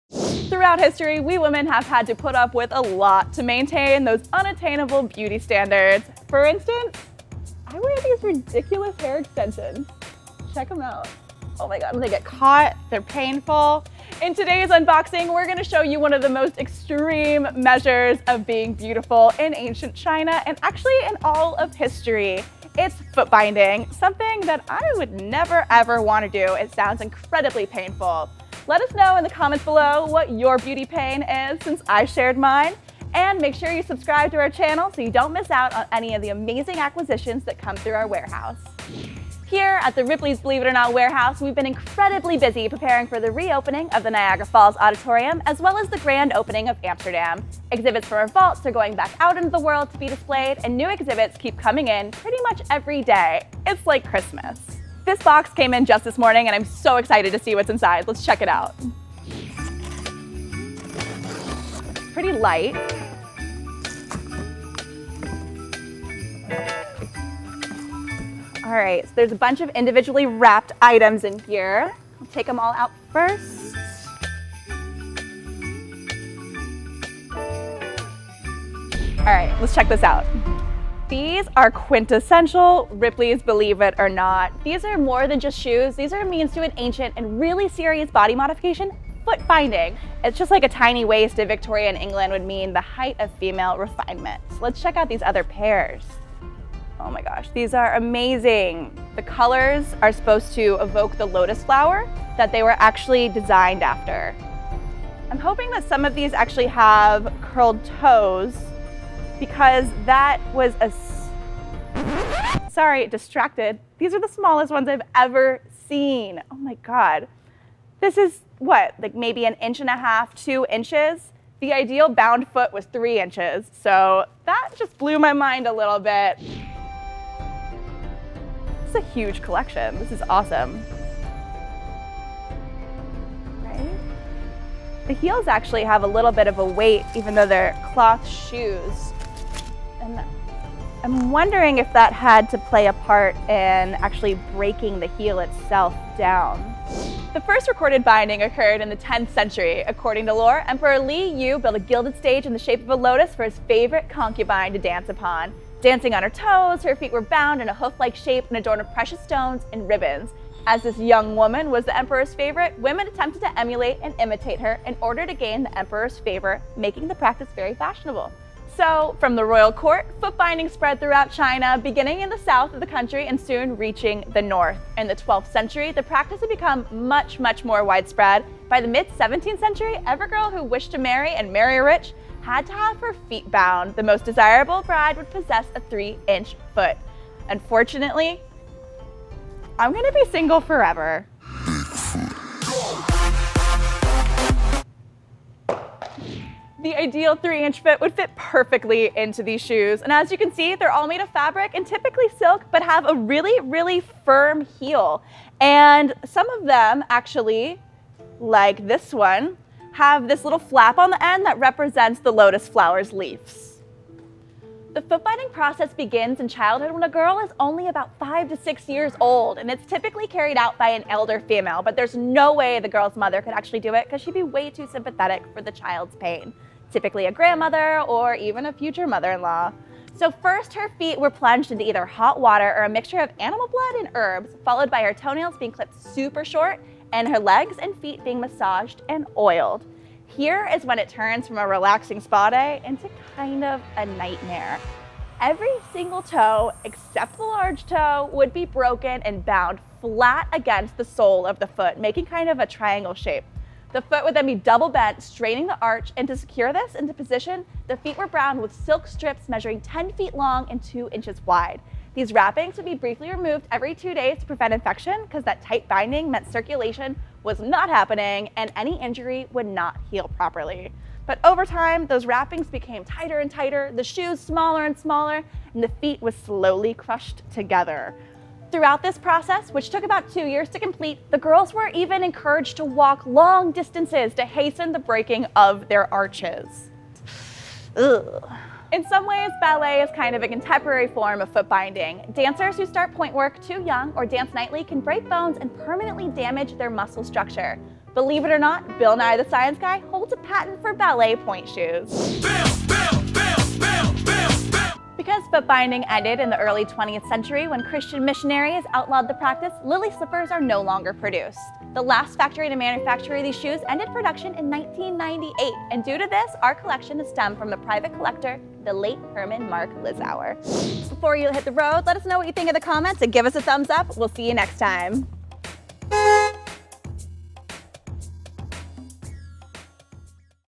Chinese Foot Binding: Unboxing Lily Slippers
For today's unboxing, we are showing you something quintessential Ripley’s: Chinese Lily Slippers, also known as Lotus Shoes.

Lily slippers are quintessential Ripley’s…These are more than just shoes! These are a means to an ancient and very serious body modification—foot binding. Binding became a popular means of displaying status in China. The first recorded foot binding occurred in the 10th century. According to lore, Emperor Li Yu built a gilded stage in the shape of a lotus flower for his favorite concubine to dance upon. Dancing on her toes, her feet were bound in a hoof-like shape and adorned with precious stones and ribbons.

As this young woman was the emperor’s favorite, women attempted to imitate her to gain the emperor’s favor—making the practice fashionable. So, from the royal court, foot binding spread throughout China, beginning in the south of the country and soon reaching the north.

In the 12th century, the practice had become much more widespread, and by the mid-17th century, every girl who wished to marry—and marry rich—had her feet bound.

The most desirable bride possessed a 3-inch foot! That perfect 3-inch foot would fit perfectly into these shoes! As you can see, these lily slippers are made of fabric, often silk, but have a very firm heel. The colorful nature of these shoes, and the curled tips found on some, were meant to evoke the image of the Lotus Flower.

The Lotus feet binding process began in childhood, when a girl was about 5 or 6, and was typically carried out by an elder female—just not the girl’s mother as she would possibly be too sympathetic to her child’s pain.

First, her feet were plunged into either hot water or a mixture of animal blood and herbs, followed by her toenails being clipped very short and the being massaged and oiled.

All of the toes except the big toes would be broken and bound flat against the sole of the foot, making a triangle shape. The foot we be bent double, staining the arch. To secure this position, the feet were bound with silk strips measuring ten feet long and two inches wide. These wrappings would be briefly removed every two days to prevent infection (the tight binding meant circulation was cut off and any injury wouldn’t heal properly).

Sometimes, excess flesh would be cut away or even encouraged to rot. Over time, the wrappings became tighter, and the shoes smaller as the heel and sole were slowly crushed together.

Throughout this process, which took two years to complete, the girls were encouraged to walk long distances to hasten the breaking of their arches.